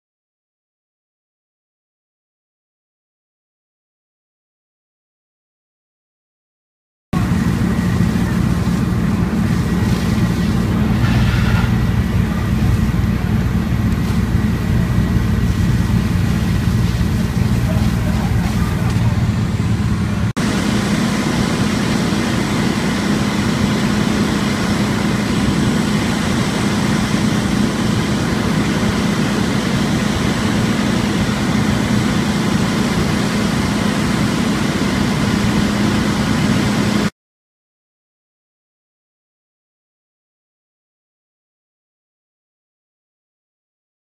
Southington House Fire
Three residents were hospitalized after a fire broke out on the second floor of a home on Church Street in Southington. In total five were displaced by the fire. March 18, 2015